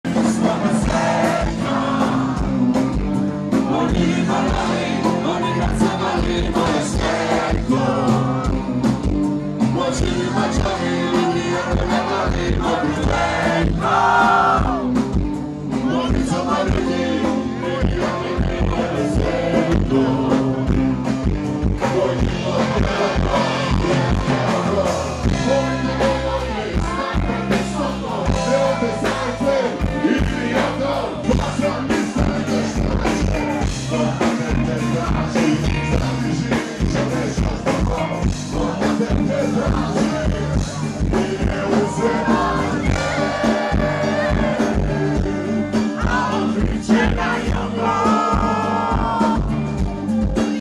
Koncert TBF-a i doček Nove godine na Zvončacu
Split, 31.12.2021 - Koncert TBF-a i doček Nove godine na Zvončacu. Snimio: Tom Dubravec / CROPIX